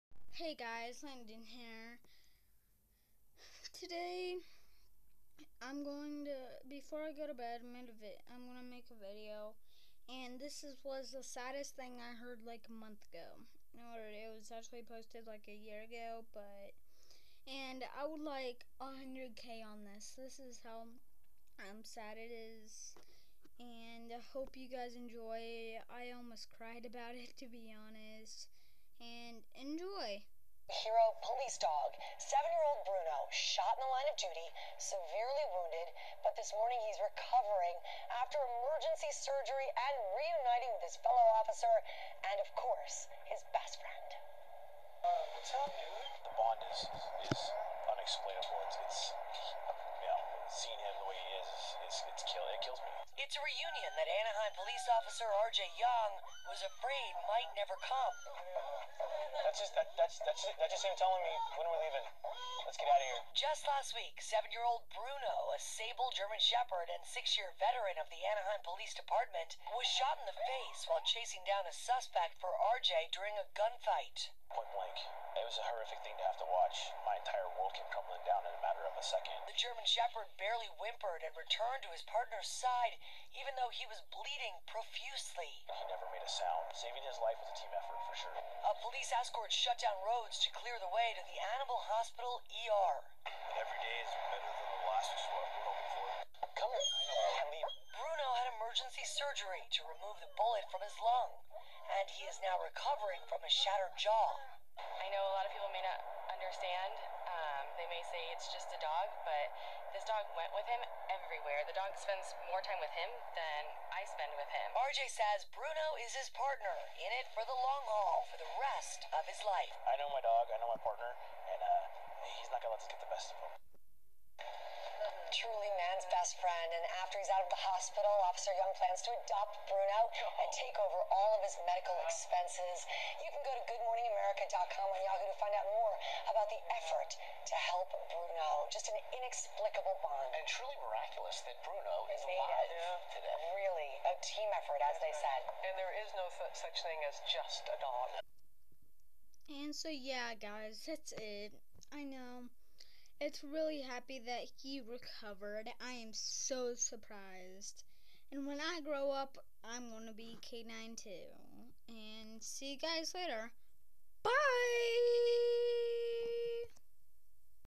Police dog shot in line of duty reunited with cop
Bruno the German shepard was shot in the face while chasing down a suspect